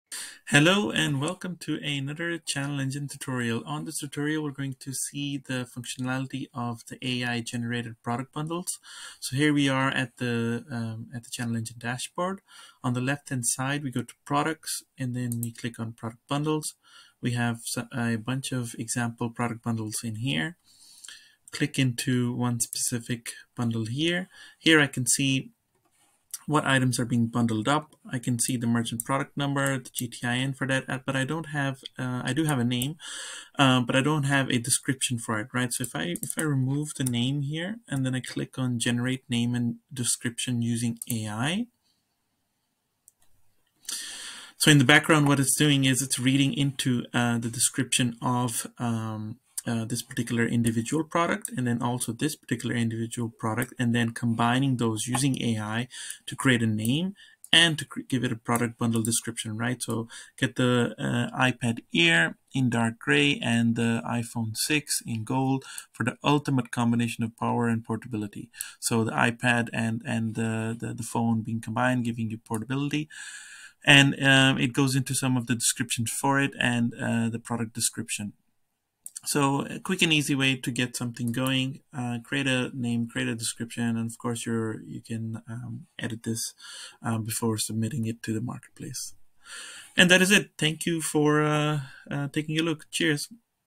Tutorial: How to use AI-Powered Product Bundling on Marketplaces | ChannelEngine
Imagine generating attractive bundle names and descriptions in seconds.💡 That is now possible! With our new 💥 AI-Powered Product Bundling , you can effortlessly optimize bundle content to attract more customers.

But what are the benefits of using virtual product bundles for marketplace sellers?
✔️Gain competitive advantage
✔️Increase perceived value
✔️Unlock cross-selling opportunities
✔️Enhance customer satisfaction
✔️Optimize inventory management
✔️Create cost-effective promotions
✔️Gain profitability of lower-priced items

In addition, this feature works seamlessly across all your marketplaces, giving you a powerful tool to boost sales and stand out from the competition. 💪✨

▶️ Watch the video where Jahangeer Iqbal walks you through our first AI-Powered solution and start using it today!